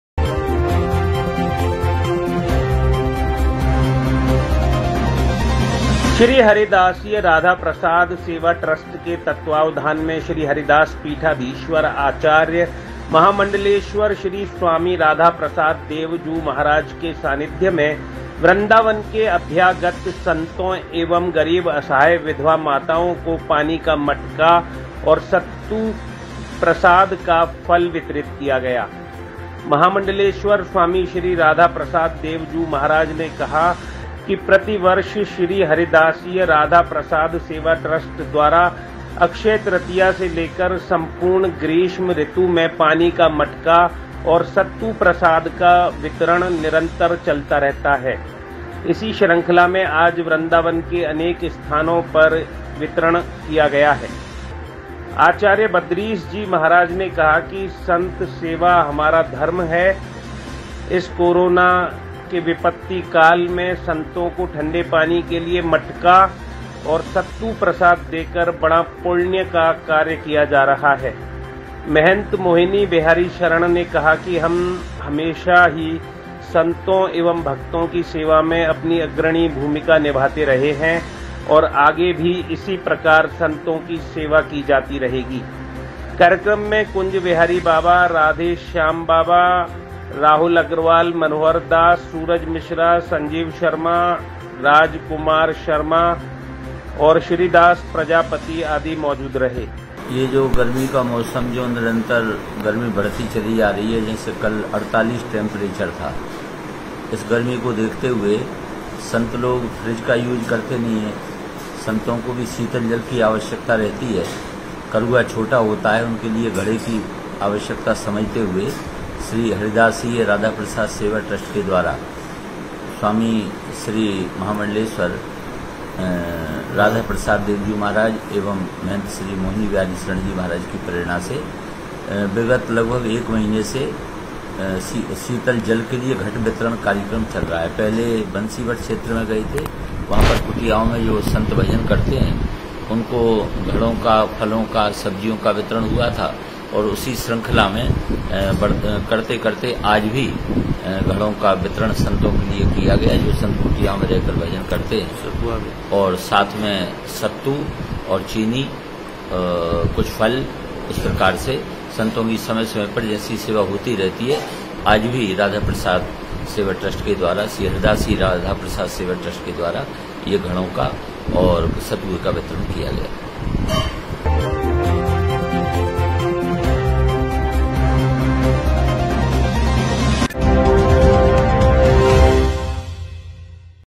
गरीब असहाय विधवा माताओं को पानी का मटका और सत्तू प्रसाद फल का किया वितरण | TTI NEWS
भारत के उत्तर प्रदेश राज्य के मथुरा जनपद की धर्म नगरी वृंदावन में आए दिन धार्मिक आयोजन होते रहते हैं। इसी श्रंखला में हुए एक आयोजन की यह कवरेज है। गर्मी में सत्तू मटका और पानी आदि की व्यवस्था गरीबों के लिए किए जाने के कार्य की यह कवरेज लोगों के लिए प्रेरणा स्रोत है।